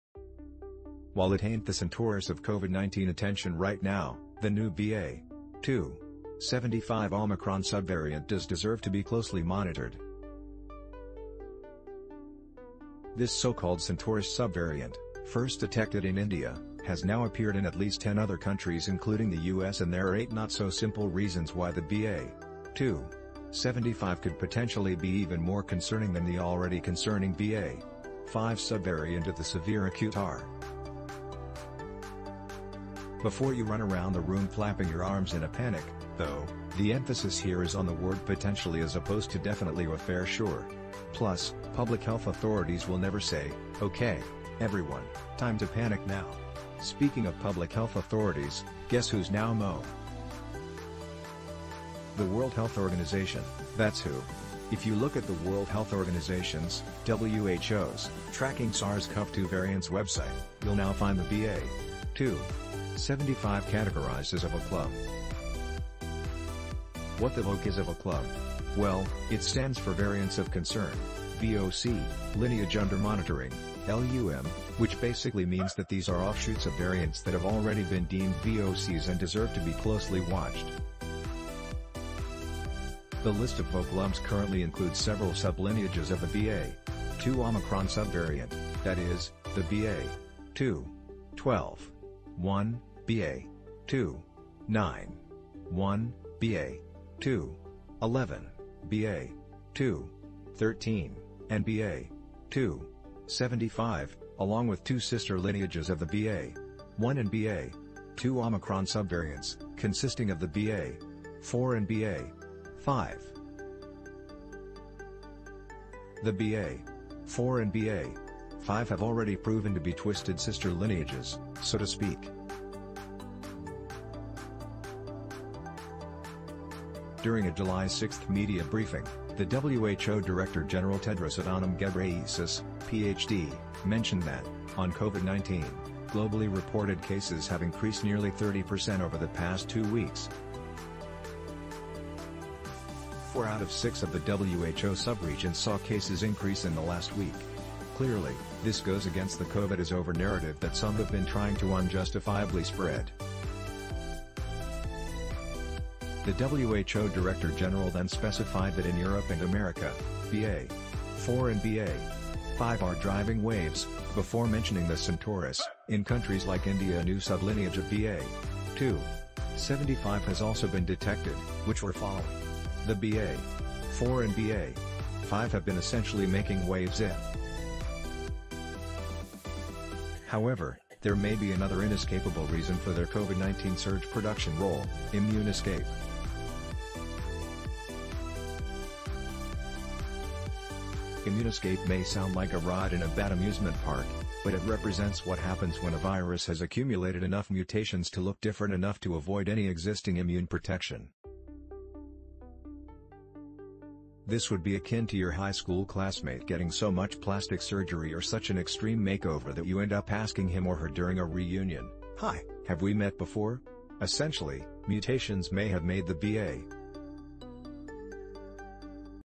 New BA 2 75 Omicron Subvariant Here’s The ‘Centaurus’ Covid 19 Coronavirus
While it ain’t the “Centaurus” of Covid-19 attention right now, the new BA.2.75 Omicron subvariant does deserve to be closely monitored. This so-called “Centaurus” subvariant, first detected in India, has now appeared in at least 10 other countries including the U. S. And there are eight-not-so-simple reasons why the BA.2.75 could potentially be even more concerning than the already concerning BA.5 subvariant of the severe acute respiratory syndrome coronavirus 2 (SARS-CoV-2). Before you run around the room flapping your arms in a panic, though, the emphasis here is on the word “potentially” as opposed to “definitely” or “fer sure.” Plus, public health authorities will never say, “OK, everyone, time to panic now.”Speaking of public health authorities, guess who’s now monitoring the BA.2.75? The World Health Organization, that’s who. If you look at the World Health Organization’s (WHO’s) Tracking SARS-CoV-2 variants website, you’ll now find the BA.2.75 categorized as a VOC-LUM. What the VOC is a VOC-LUM? Well, it stands for Variants of Concern (VOC) Lineage Under Monitoring (LUM), which basically means that these are offshoots of variants that have already been deemed VOCs and deserve to be closely watched. The list of VOC-LUM’s currently includes several sub-lineages of the BA.2 Omicron subvariant (that is, the BA.2.12.1, BA.2.9.1, BA.2.11, BA.2.13, and BA.2.75) along with two sister lineages of the BA.1 and BA.2 Omicron subvariants (consisting of the BA.4 and BA.5). The BA.4 and BA.5 have already proven to be twisted sister lineages, so to speak. During a July 6 media briefing, the WHO Director-General Tedros Adhanom Ghebreyesus, PhD, mentioned that, “On Covid-19, globally reported cases have increased nearly 30% over the past two weeks. Four out of six of the WHO sub-regions saw cases increase in the last week.” Clearly, this goes against the Covid is over narrative that some have been trying to unjustifiably spread. The WHO Director-General then specified that “In Europe and America, BA.4 and BA.5 are driving waves,” before mentioning the Centaurus: “In countries like India a new sub lineage of BA.2.75 has also been detected, which we’re following.”The BA.4 and BA.5 have been essentially making waves in part because they’ve proven more transmissible than previous versions of the virus. However, there may be another inescapable reason for their Covid-19 surge production role: immune escape. Immune escape may sound like a ride in a bad amusement park, but it represents what happens when a virus has accumulated enough mutations to look different enough to avoid any existing immune protection. This would be akin to your high school classmate getting so much plastic surgery or such an extreme makeover that you end up asking him or her during a reunion, “hi, have we met before?” Essentially, mutations may have made the BA.